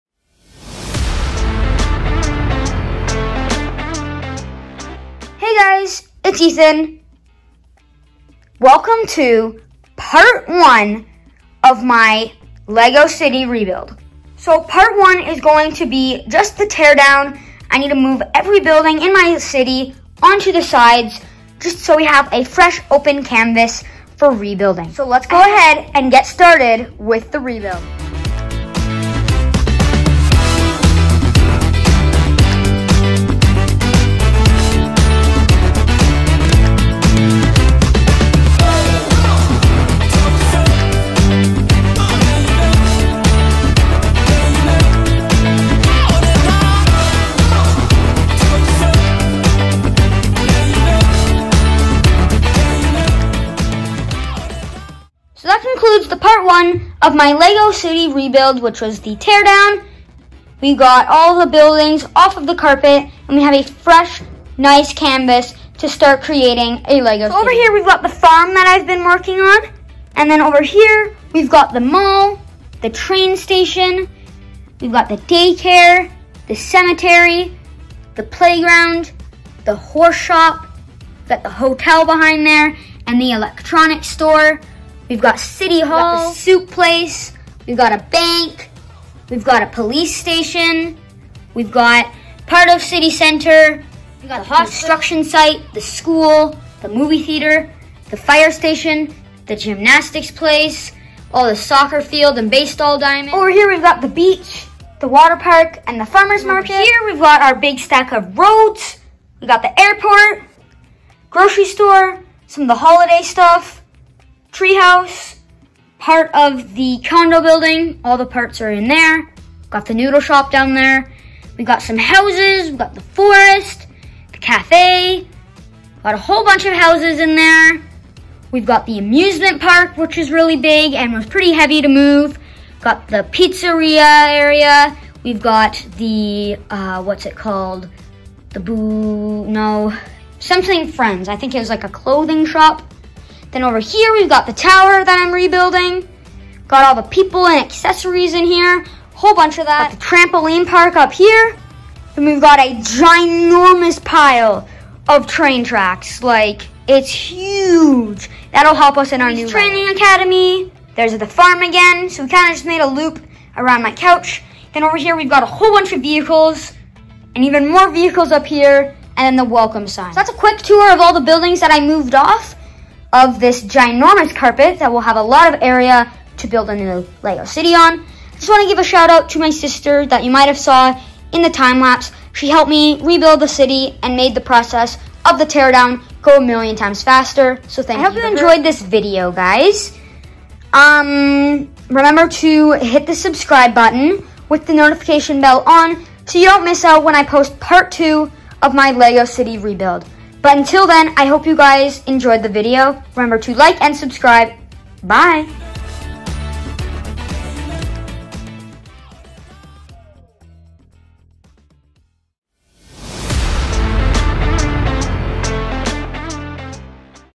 City Rebuild Part 1: The Tear Down
In this video, I’m going to start part one of my lego city rebuild, which is the tear down. I’m going to be taking you guys along the journey through this rebuild series.